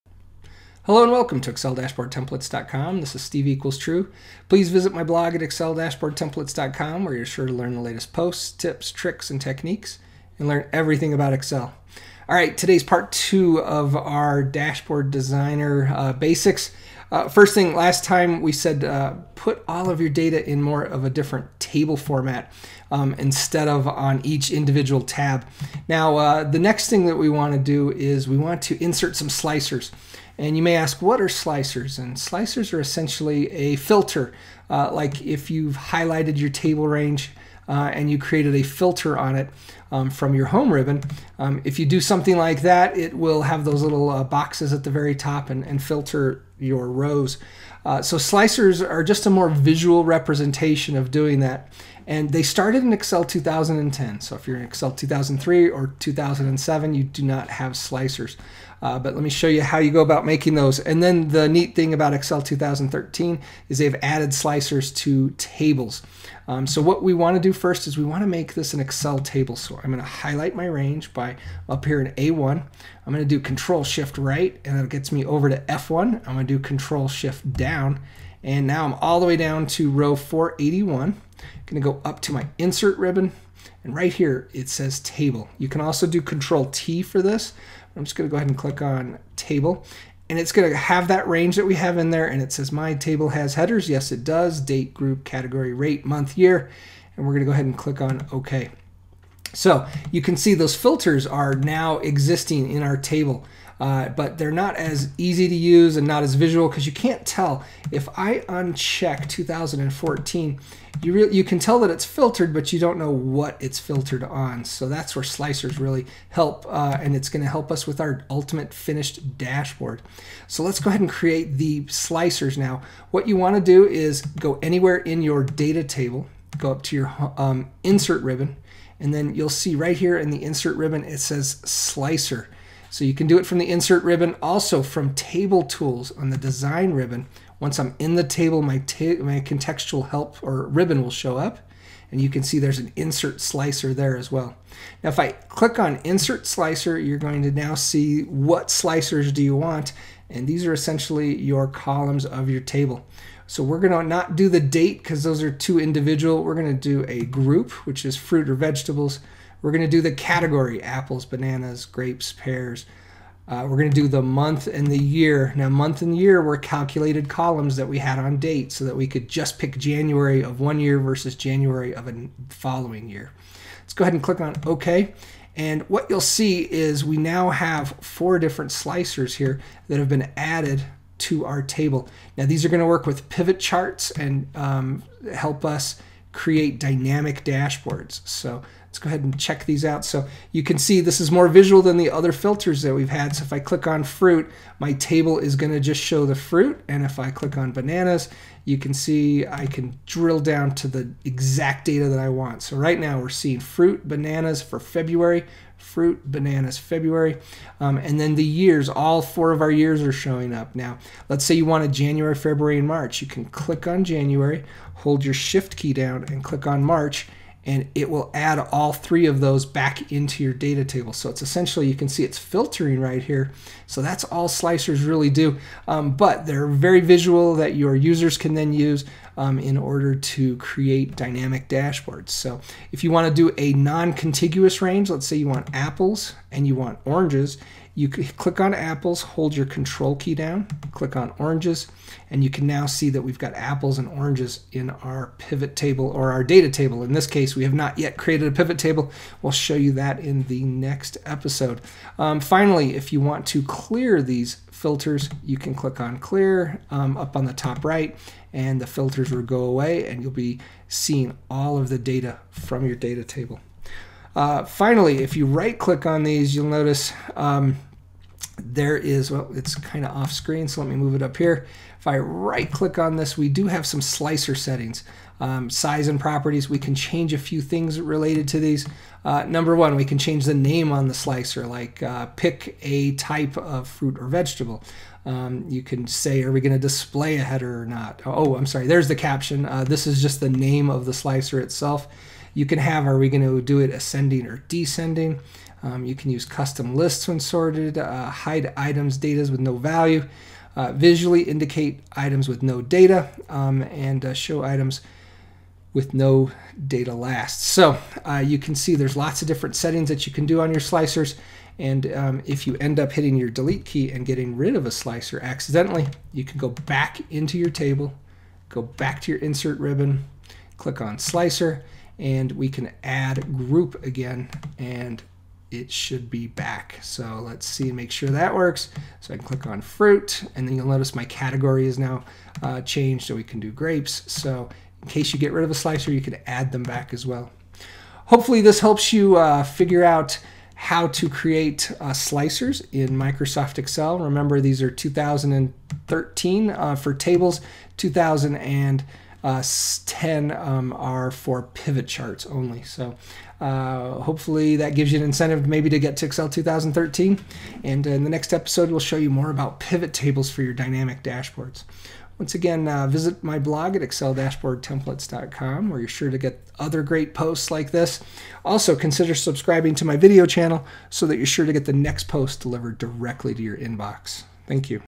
How-to Create Excel Slicers for Your Dynamic Dashboard
Slicers are great for letting users dynamically control an Excel Dashboard Chart.  In this video, you will learn how to create Slicers in preparation of creating a Dynamic Chart.